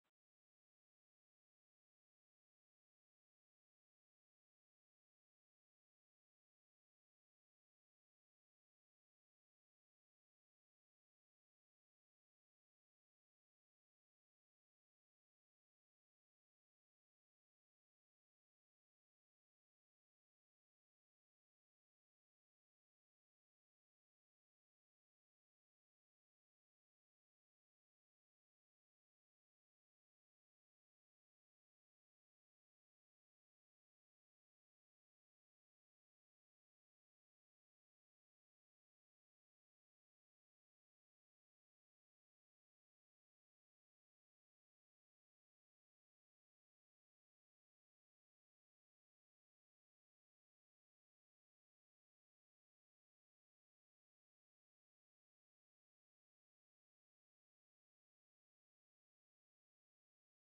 Compra Piso en Cornellà de Llobregat - PISO CON TERRADO COMUNITARIO
Piso en zona Almeda de 2 habitaciones semirreforma... Para más información visita